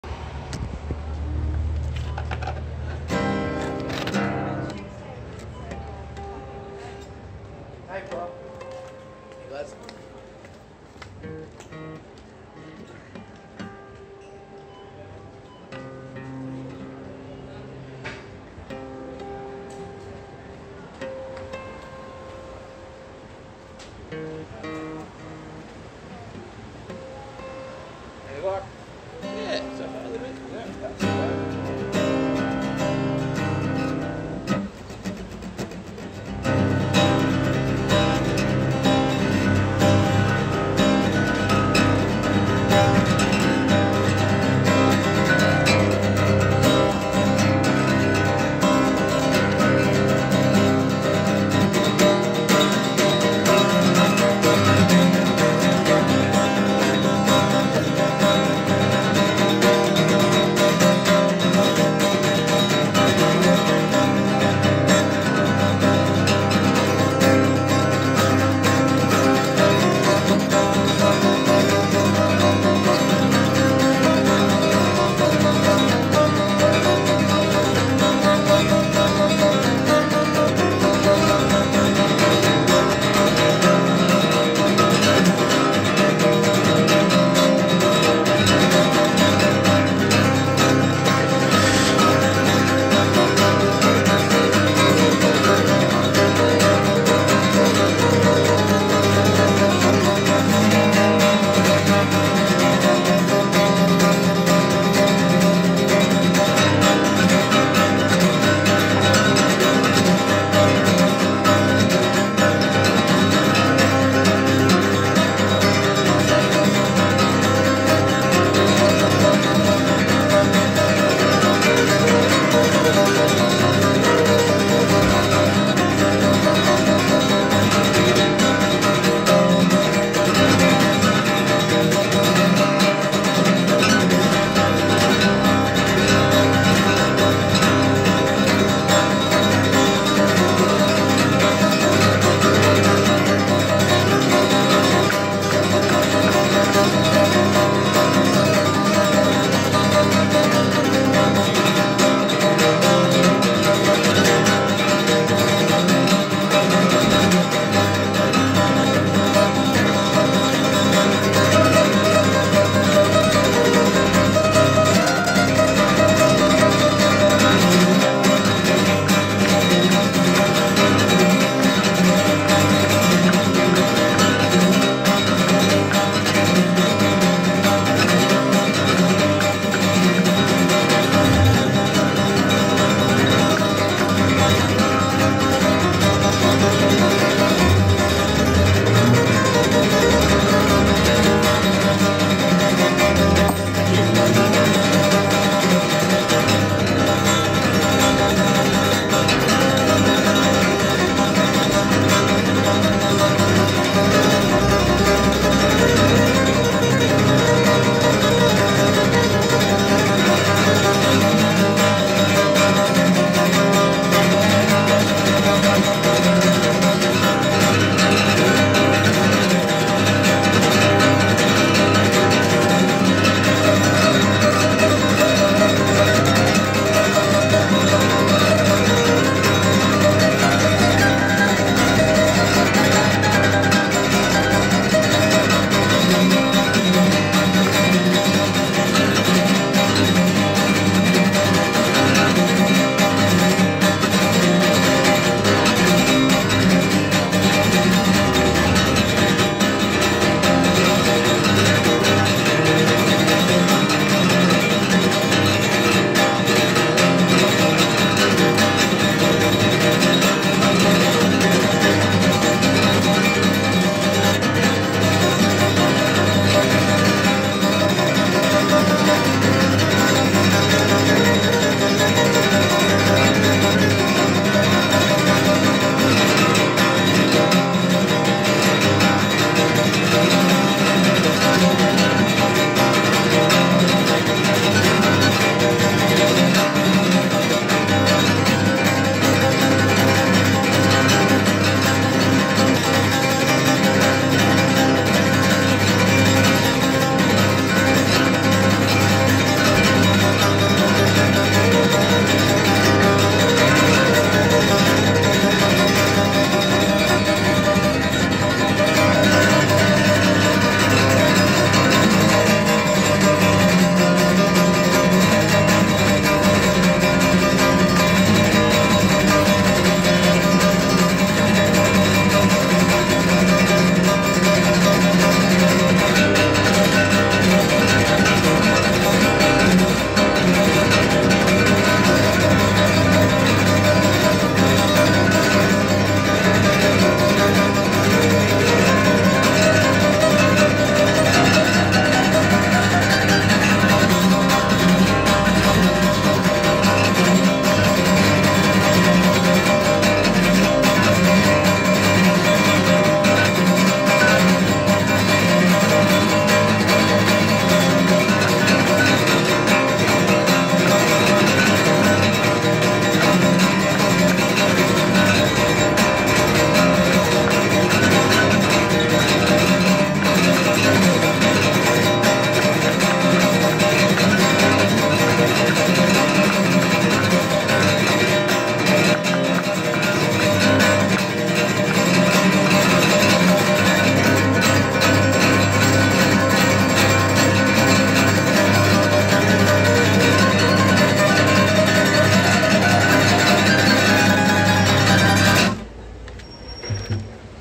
GREAT GUITAR STREET PERFORMANCE . AUSTRALIA.MELBOURNE
NEW TALENTED GUITARIST, START SETUP EASY GOING STAND SIDE WALKAWAY, BUT HES PERFORMANCE TAKE YOU TOO JOY TRIP IN YOUR DREAMS, MAKE YOUR HEART SHAKING BECAUSE IT'S MADE LEVEL YOUR BODY ENERGY AND SIMULTANEOUSLY CONNECTING YOU BODDY AND SOUL WITH AMOUNT OF LOVE FEEL & PASSIONS AND ENERGY HE SPENDS AND TROUGH GUITAR STRINGS MAKE BEAUTIFUL MELODY. MELODY OF LOVE